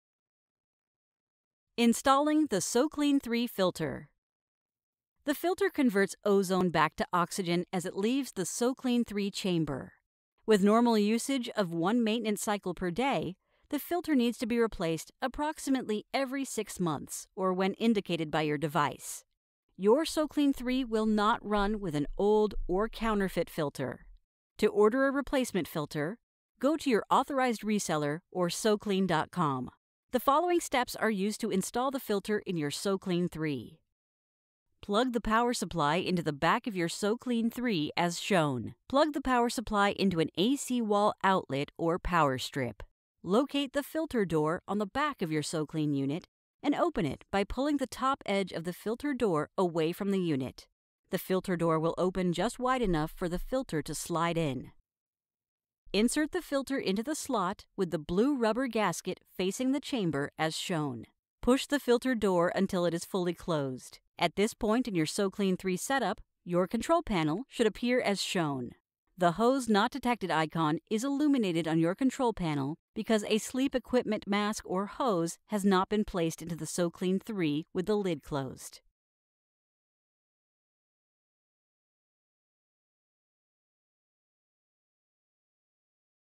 SoClean 3 Filter Instructions DirectHomeMedical
Cleaning your CPAP equipment regularly is critical to ensuring safe and effective sleep therapy; and with SoClean 3 cleaning is easy!

SoClean 3 automatically cleans and sanitizes masks, tubing and humidifier chambers while they're still connected to your CPAP machine. Using a natural cleaning process, SoClean 3 effectively destroys 99.9% of mold, bacteria and viruses without the use of liquids or harsh chemicals.